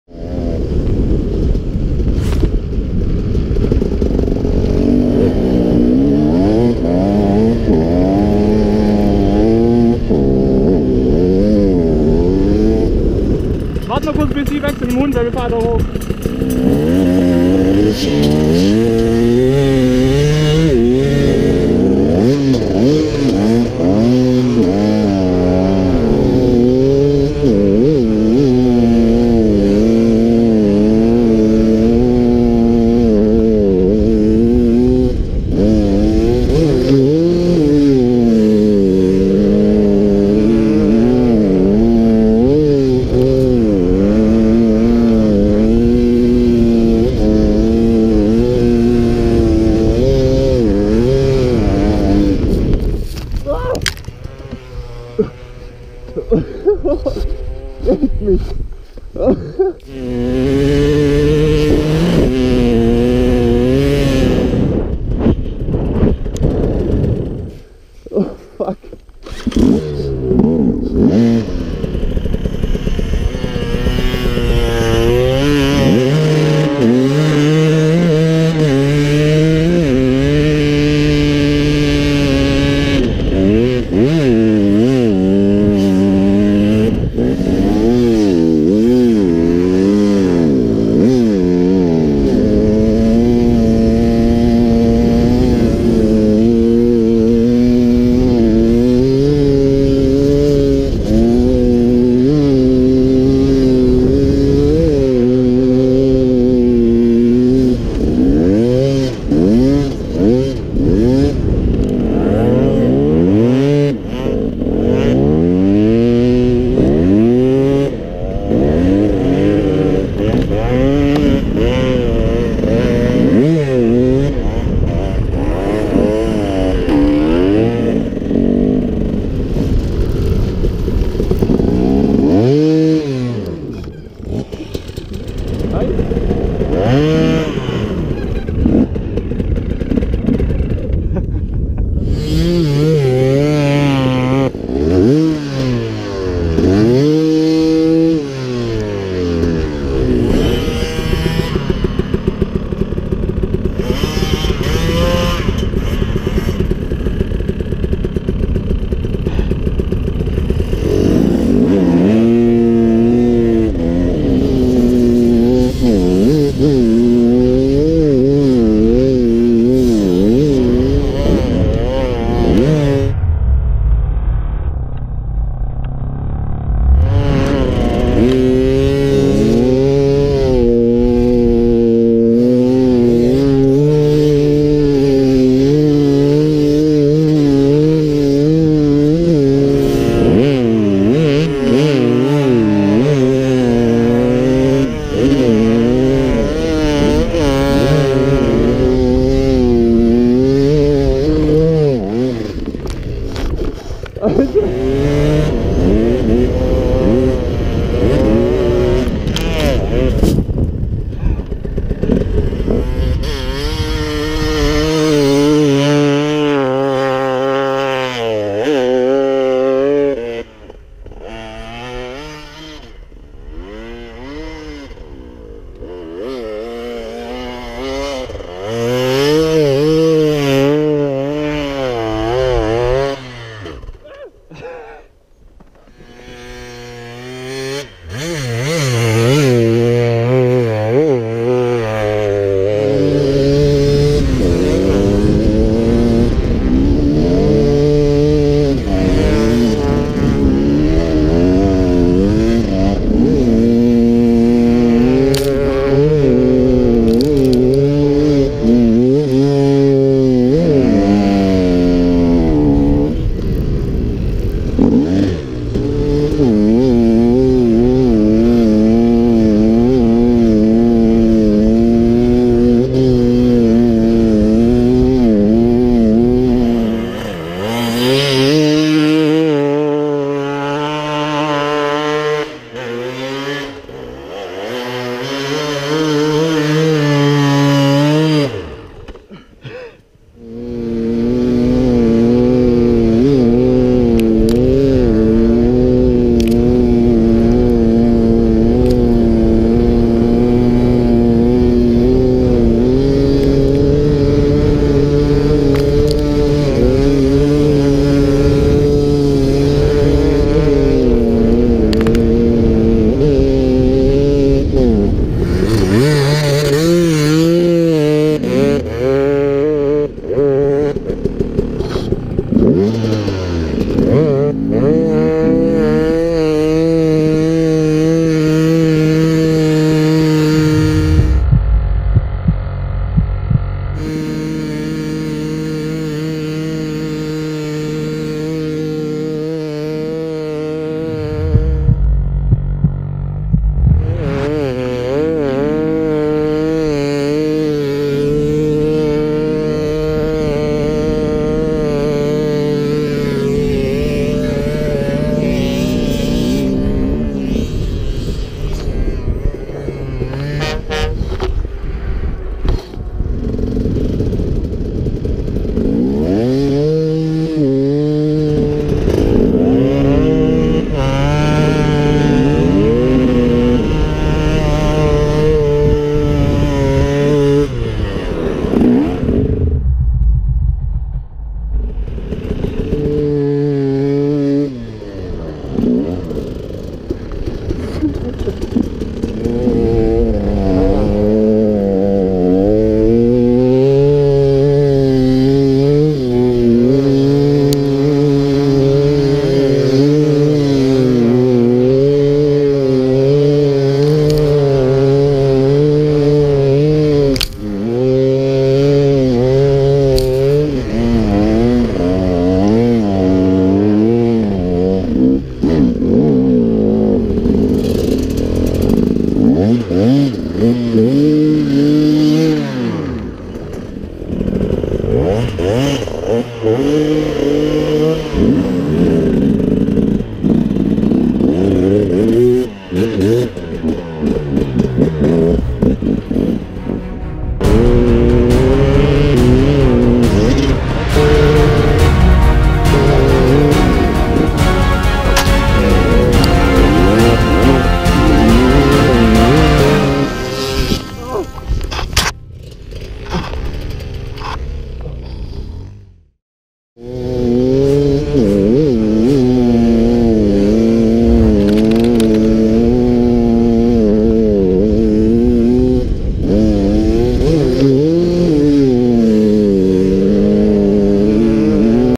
Spikes vs. No Spikes | DAMN COLD Enduro day
Sick day, riding with our bikes in snow. Here you see the difference between Spikes and no spikes xD  Enjoy!